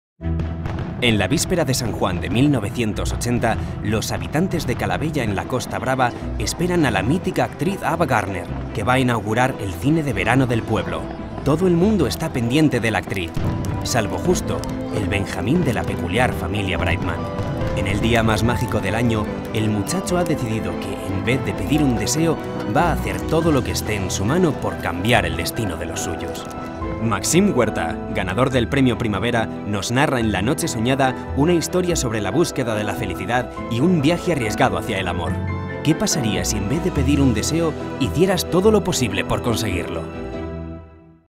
La noche soñada | Màxim Huerta | Espasa | Booktrailer
En la víspera de San Juan de 1980, los habitantes de Calabella, en la Costa Brava, esperan a la mítica Ava Gardner, que va a inaugurar el cine de verano del pueblo. Todo el mundo está pendiente de la actriz, salvo Justo, el benjamín de la peculiar familia Brightman. En el día más mágico del año, el muchacho ha decidido que, en vez de pedir un deseo, va a hacer todo lo que esté en su mano por cambiar el destino de los suyos.La noche soñada es una historia sobre la búsqueda de la felicidad.De la mano de Màxim Huerta, el lector descubrirá que el viaje más arriesgado es el que se emprende hacia el amor, tantas veces doloroso e imposible, pero con el que nunca deberíamos dejar de soñar.

¿Te ha gustado el vídeo? Comenta y compártelo ;)
¡A nosotros nos encantará leer tu opinión!